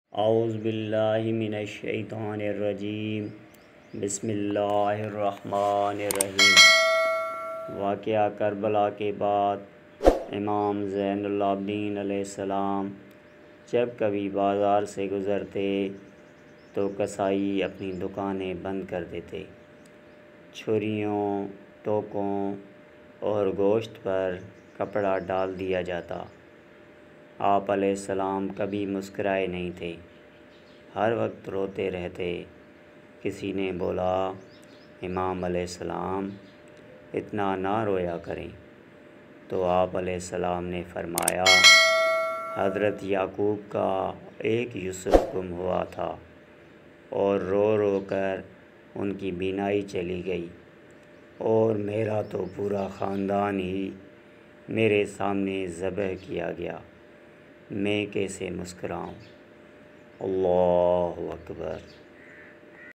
Karbala k bad Imam zainul abdin ka gham#shorts#viralshort
Viewers aj ki video waqia karbala k bad imam zainul abdin k gham h#shorts#viralshort#deedar